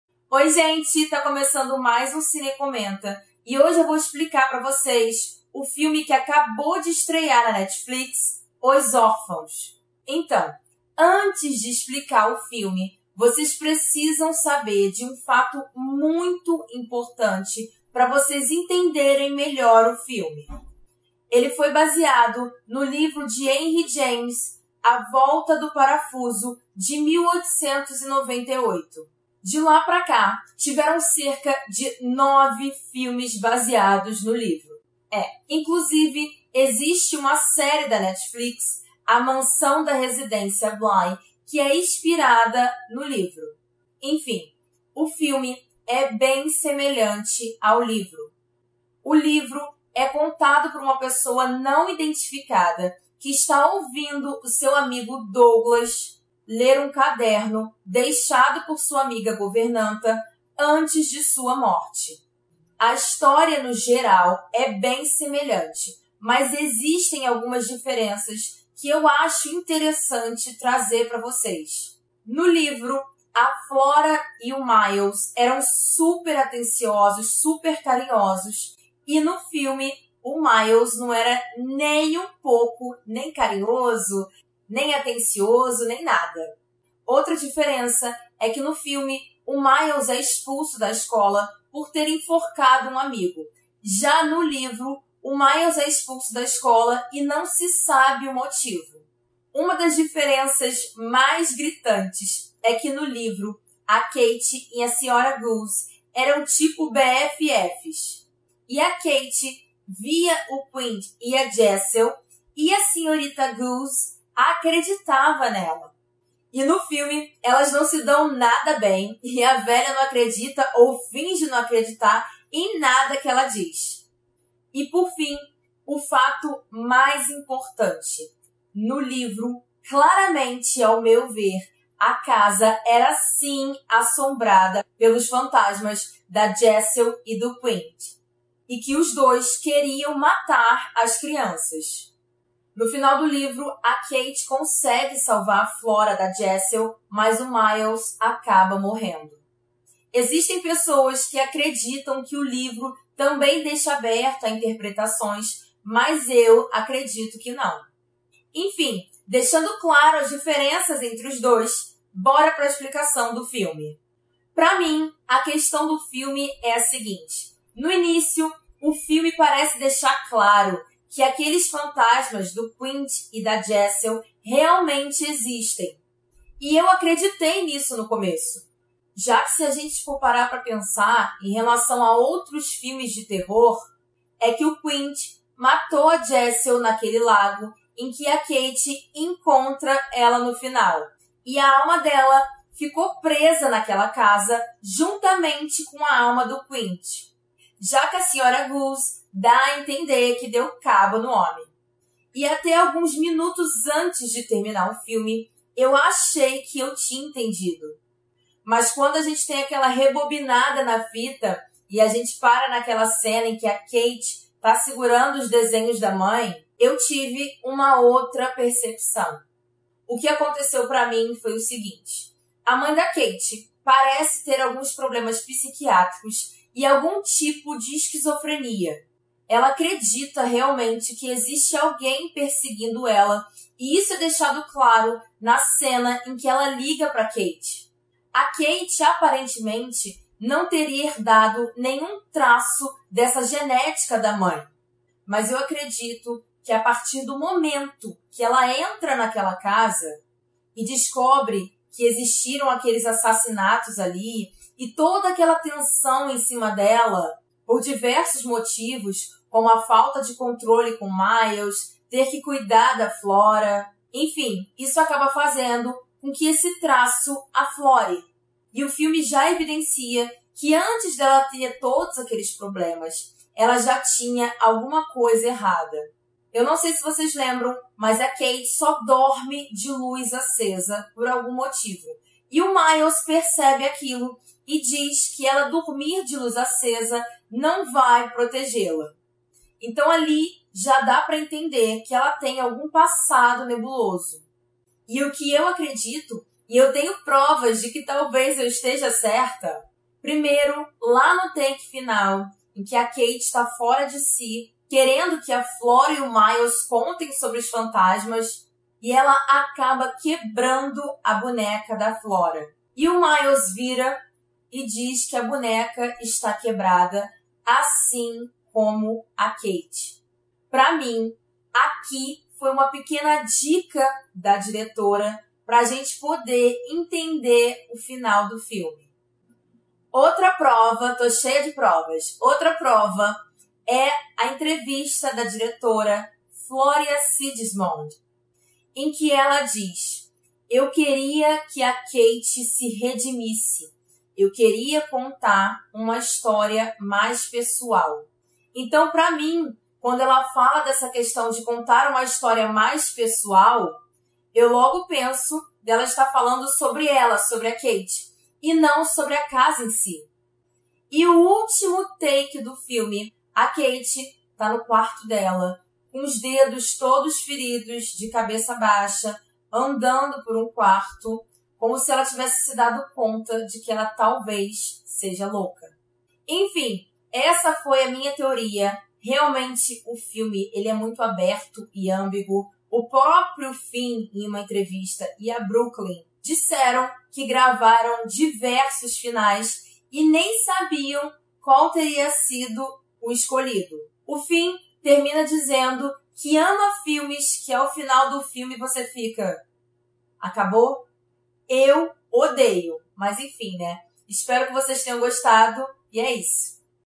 FINAL EXPLICADO OS ORFÃOS / ANÁLISE COMPLETA
Os Orfãos acaba de chegar na Netflix mas antes estava na Prime Video. O filme foi lançado nos cinemas em 2020 e foi considerado controverso por muitos. Principalmente por parte do público não ter entendido o final do filme. Então venho acabar de uma vez por todas com as dúvidas e explicar tudo para vocês!